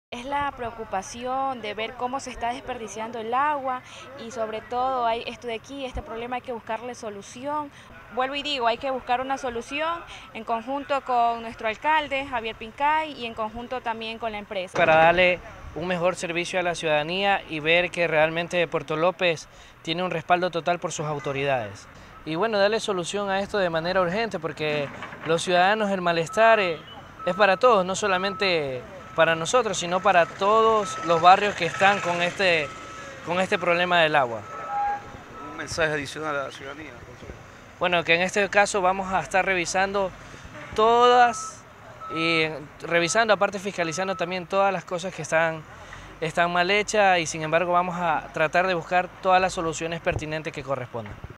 SE SOLUCIONA ROTURA DE TUBERÍAS DEL SISTEMA DE AGUA
TUBERÍAS TERMINARON SU VIDA ÚTIL